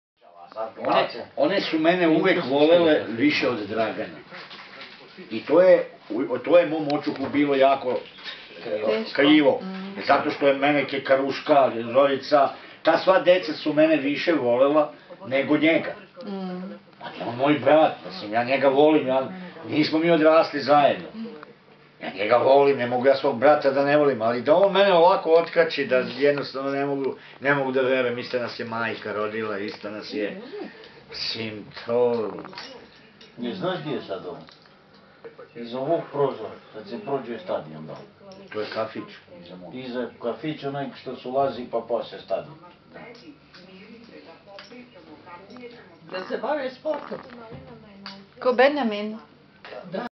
Zivica in Pancevo 2013
A short film from the home of Zivica and his wife in Pancevo.